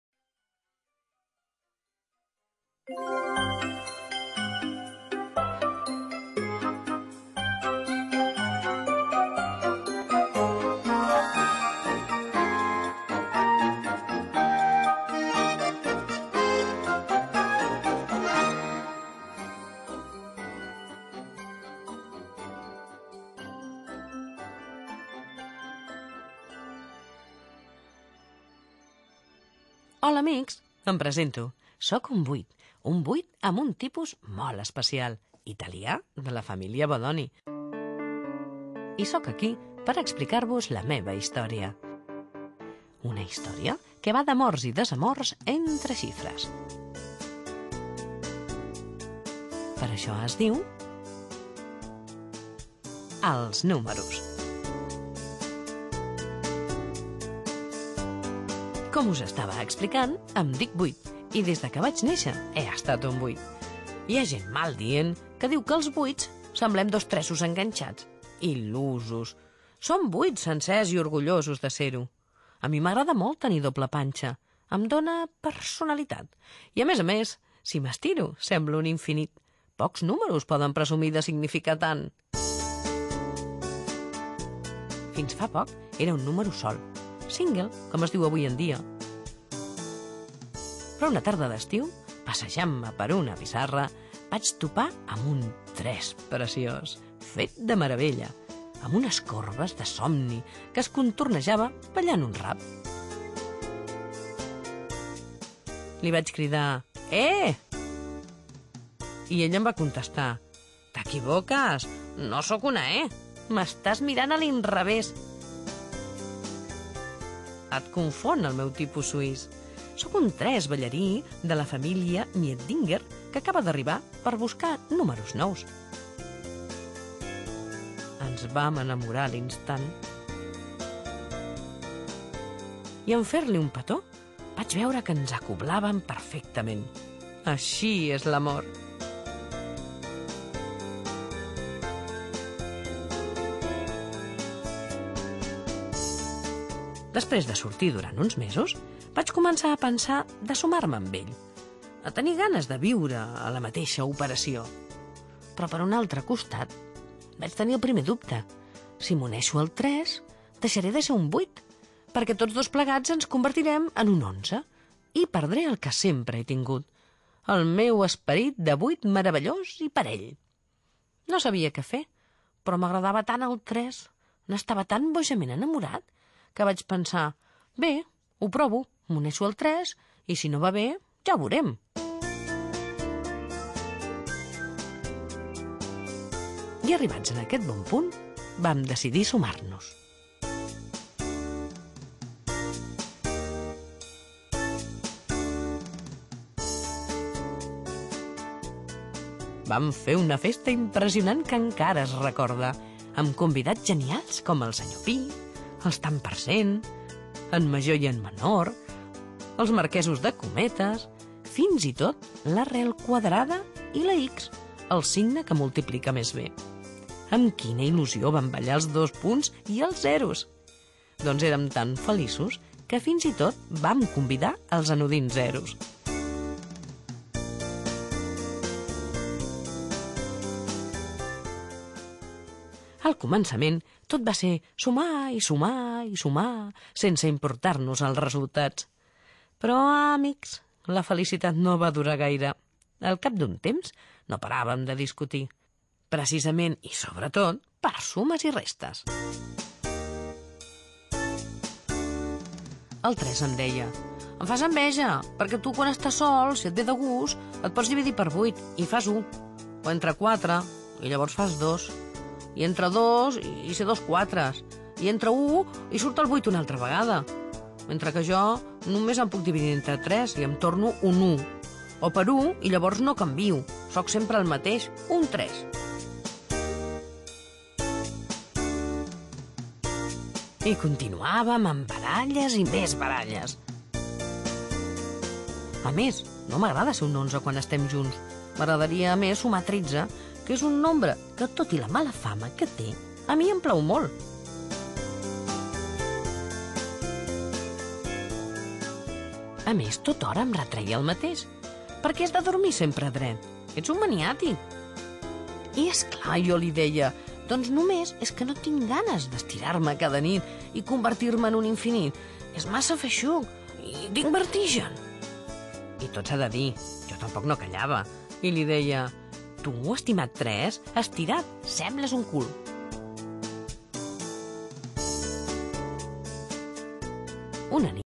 Els números
Una historia de Victoria Bemejo ilustrada por Laura Meseguer. An story by Victoria Bermejo illustrated by Laura Meseguer